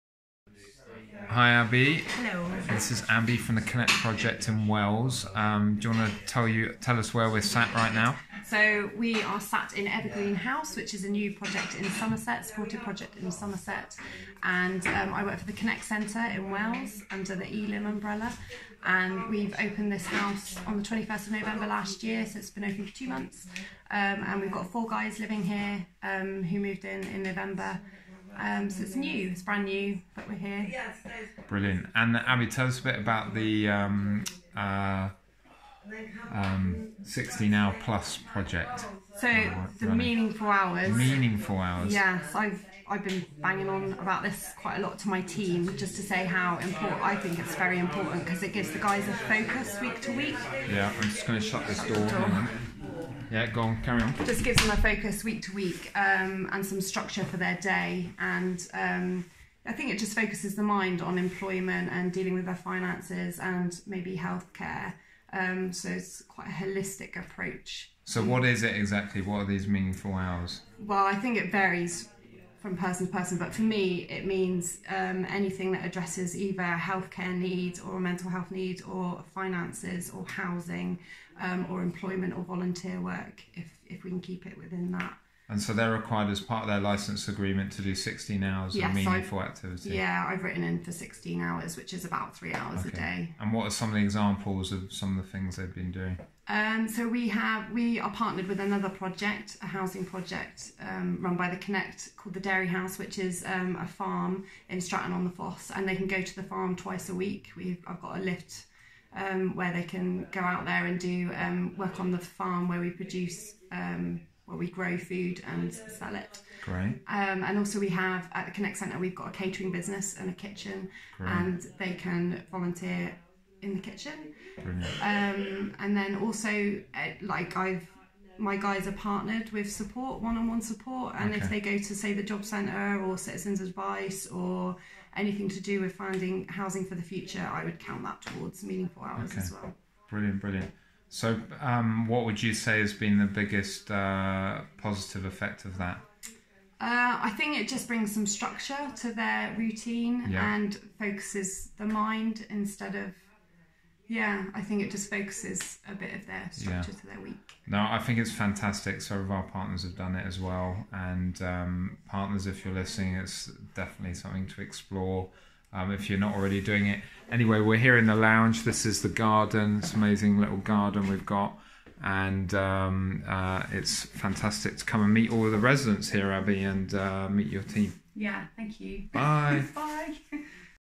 Partner, Abby from Evergreen House Elim Connect Centre Wells
Abby talks about the 'meaningful hours' requirement they have built into the licence agreement for residents as part of their holistic care.